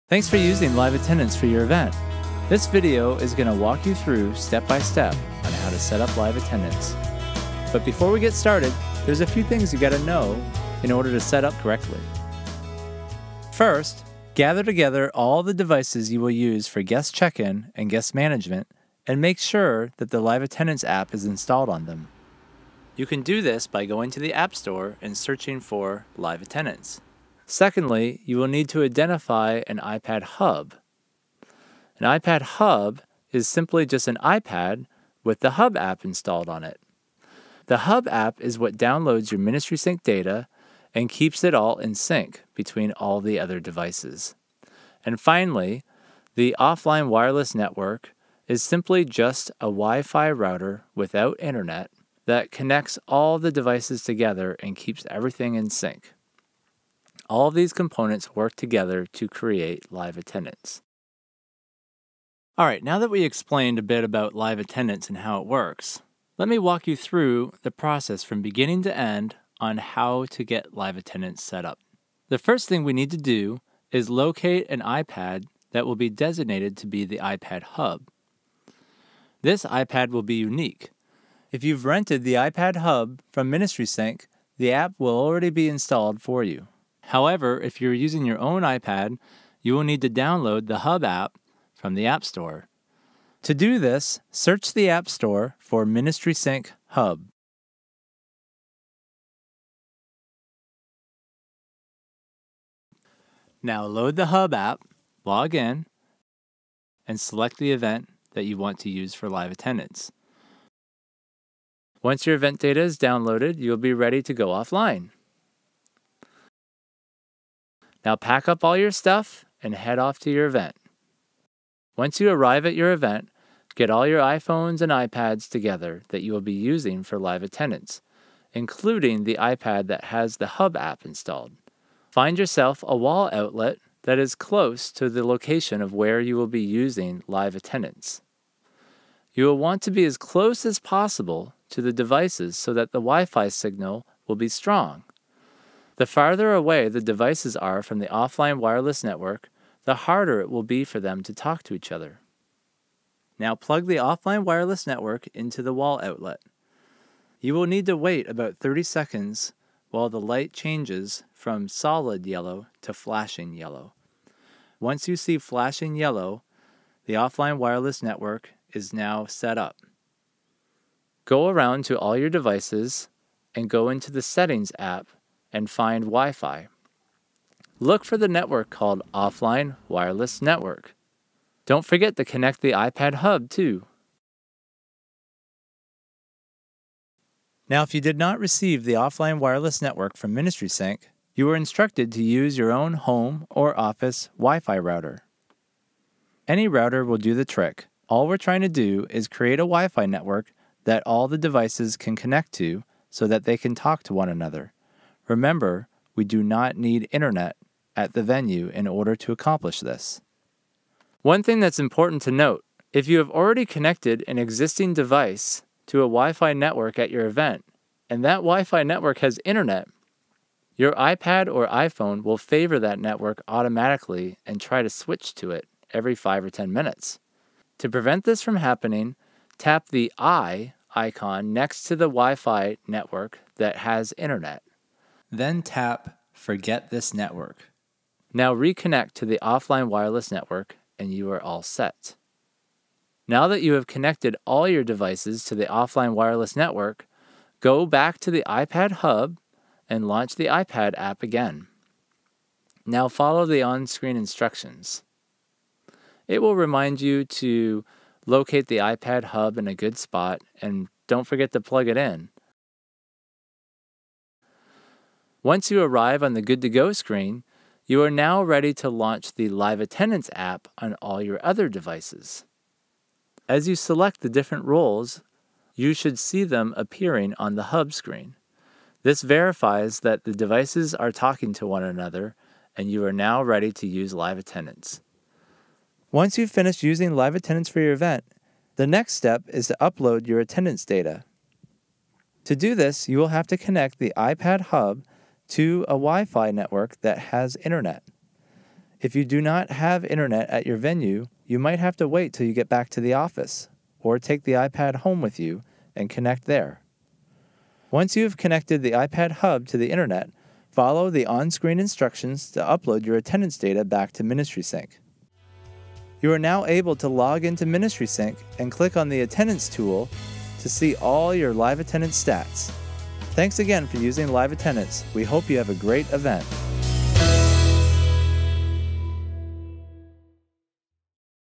Live Attendance Setup Tutorial
Get the walk-through from start to finish for how to setup Live Attendance for your event.